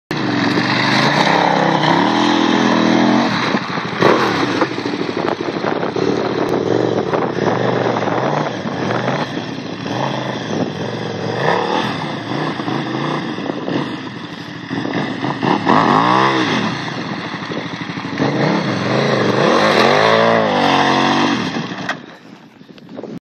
Мото развлечение зимой, мотоциклы, шипы на мото, KTM, эндуро, экстрим, отдых, веселье 4
Вот так мы с друзьями катаемся на байках в зимнюю пору, когда температура воздуха ниже 15 градусов мороза. Чтобы согреться чистим снег и дурачимся)